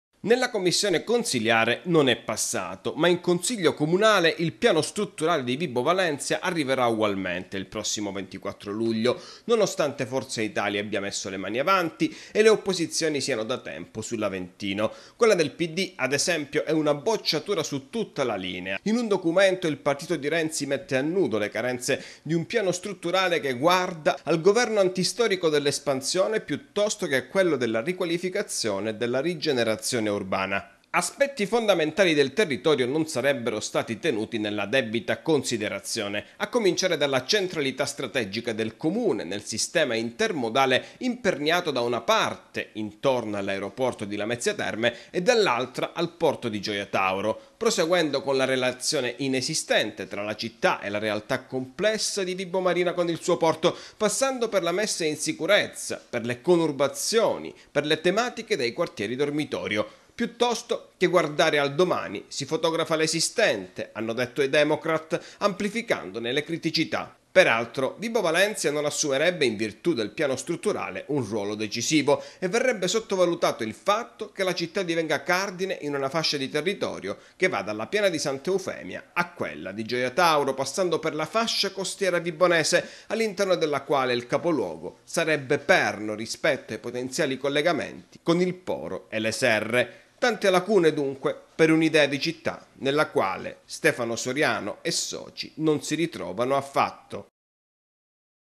VIBO: SUL PSC IL SINDACO BARCOLLA, MA NON MOLLA. DOPO FORZA ITALIA DOCUMENTO DURISSIMO PD
Il prezioso strumento urbanistico dovrebbe sbarcare presto in consiglio comunale. Ma i dubbi tra i partiti rimangono numerosi.